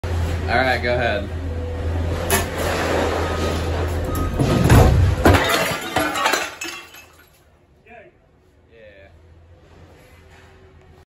Gigantic glass table shatters
One of many, MANY times we were able to destroy some stuff for fun because it was either broken or didn’t sell. This one was super satisfying.